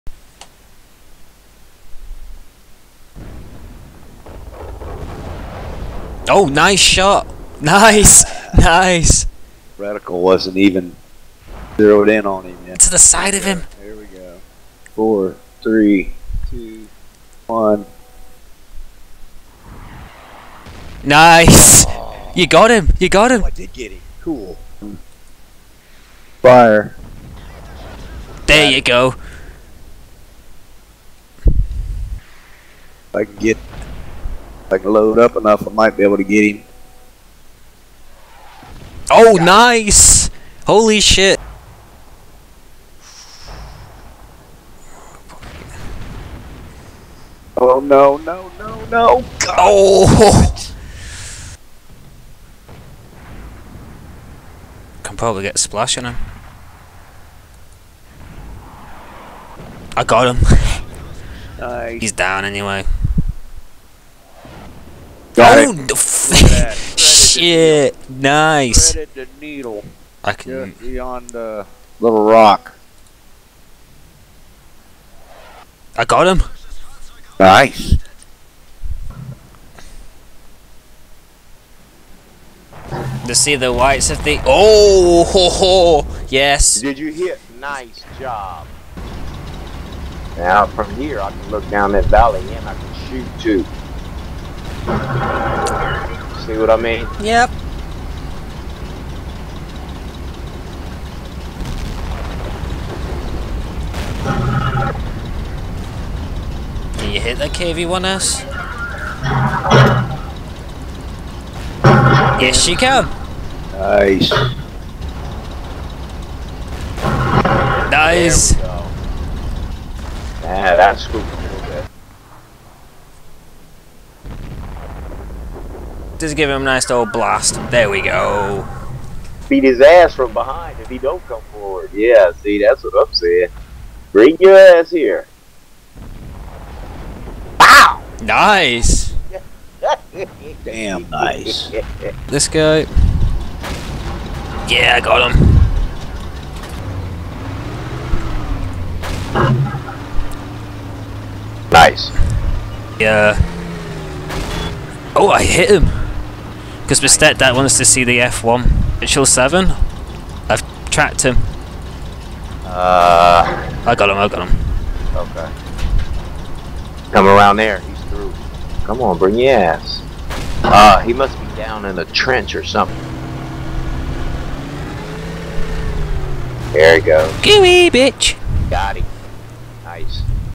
Sunday Morning Hookup November 3rd
A rough edit of the Sunday Morning Hookup with Alligator136, Athrun12341 and FreedomEagle1958